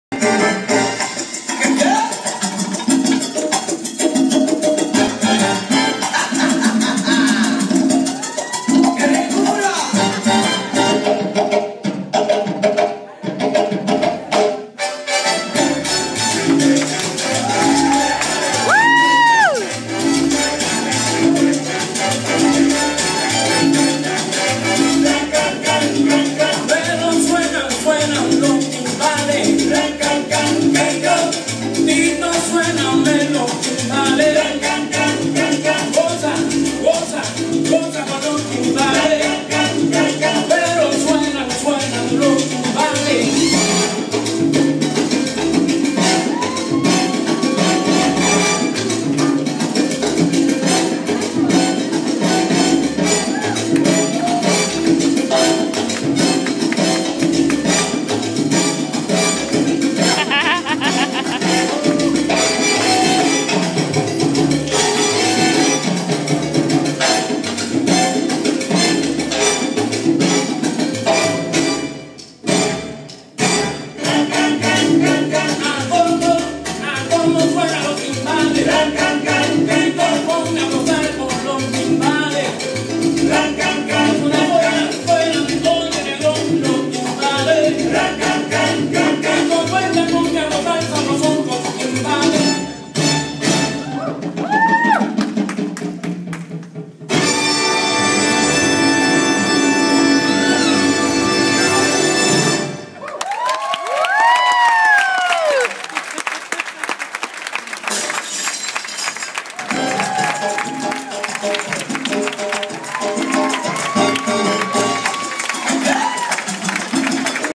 A.M PEPRALLY Mambo.MOV
Rodrigue and Diana Dancing the mambo at their Pep Rally on January 22nd